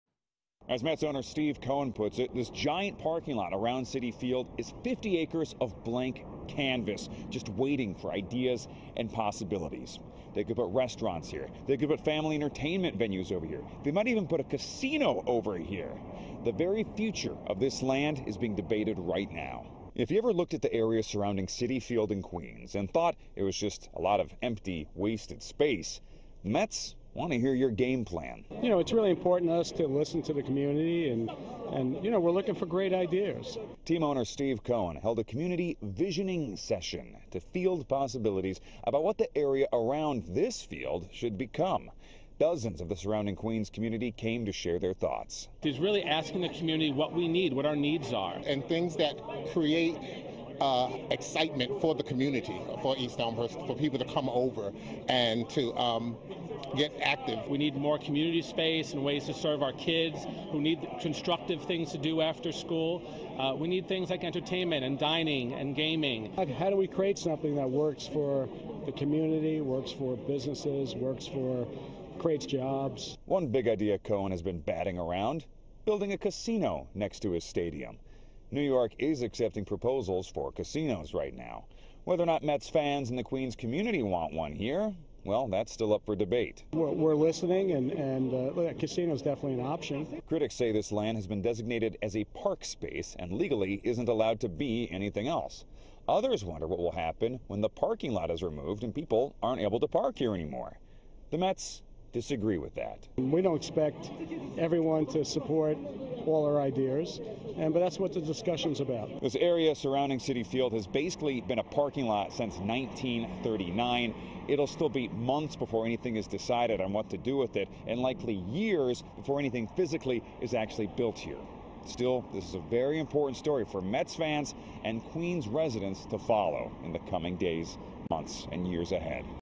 Steve Cohen asks for ideas on space around Citi Field
If you've ever attended a Mets game at Citi Field, you probably are aware of how much open space surrounds the stadium. Steve Cohen, the owner of the Mets, is currently asking the neighborhood what they want to see developed there.